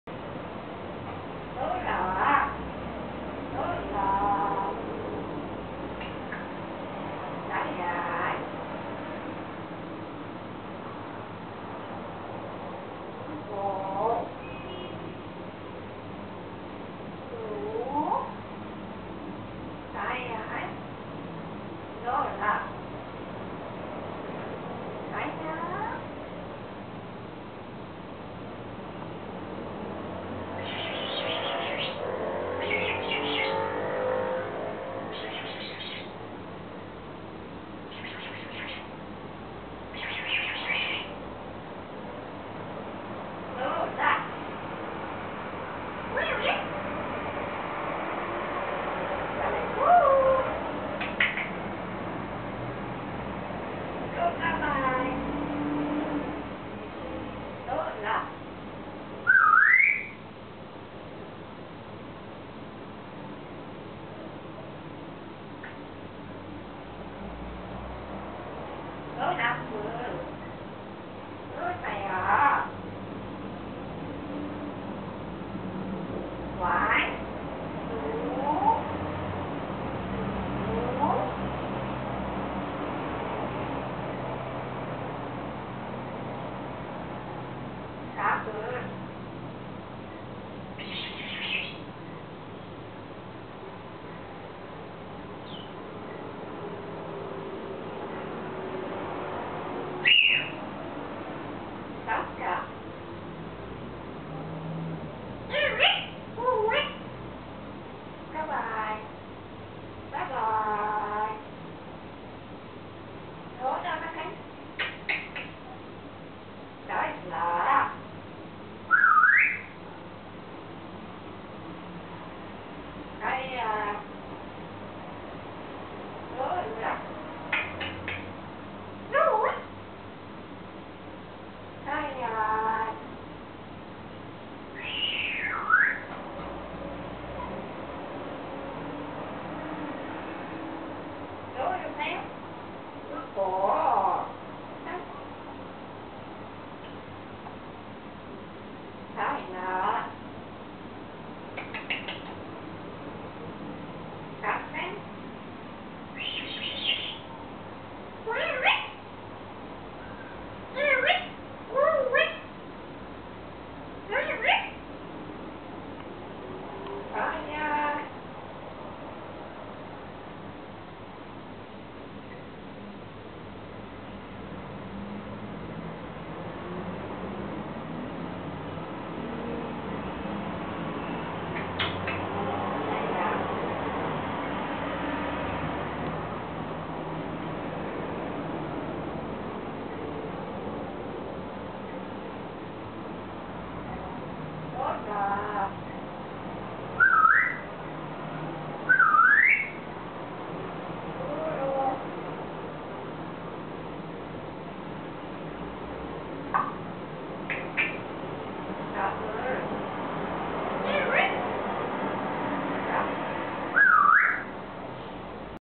Lola The Cute Little Bird
Lola (African Gray)
He Is Very Cute Love To Play Around With People:D
Soo Have Fun With This Video & Pls Do Rate Thank You:)